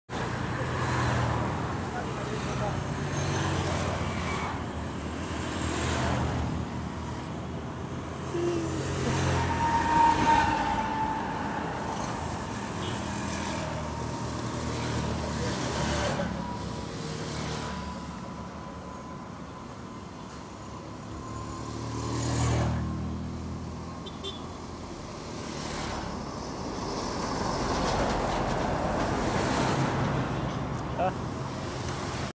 Kampala Flyover Project Progress Update: Current Queen’s Way soon to be improved
This is the current Queen’s way Road soon to be improved as part of the Kampala Flyover Project in the City.